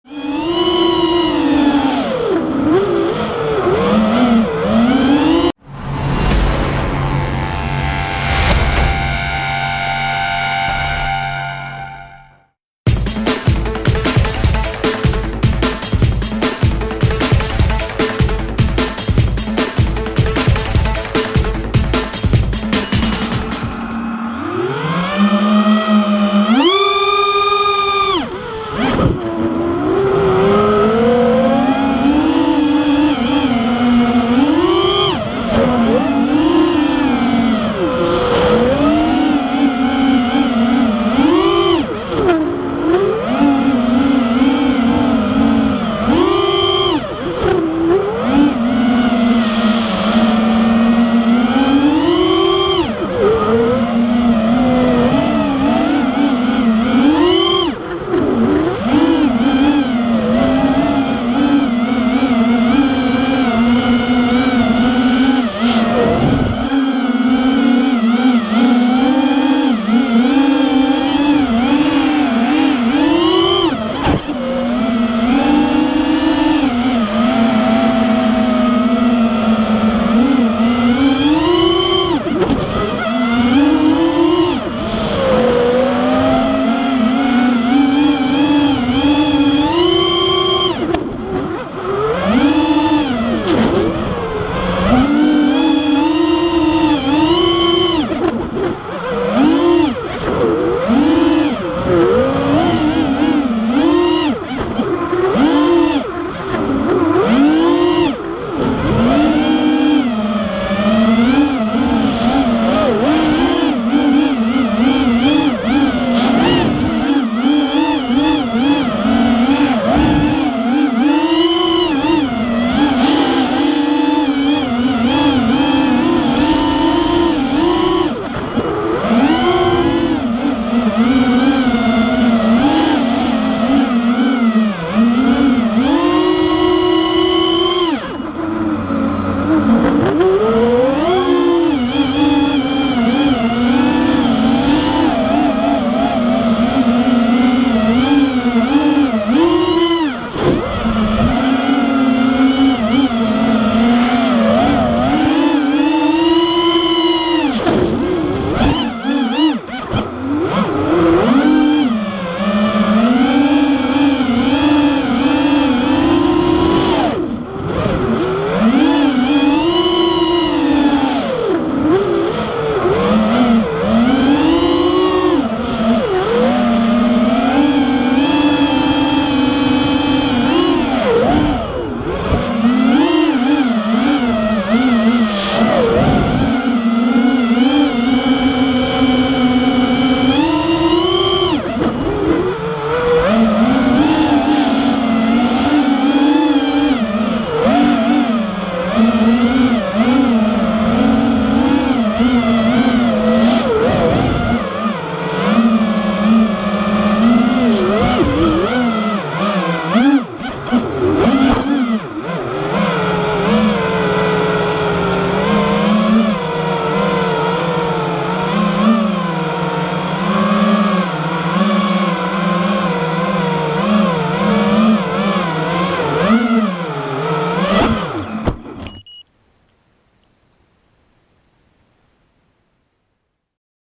Just for a run around the field F1-5 Betaflight 2.8.1
Armattan F1-5 Fc Lumenier Lux  looptime125
Emax RB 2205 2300 Dalprop 5x4.5x3 v2
LB pro 30A Multishot 14.6
Gopro protune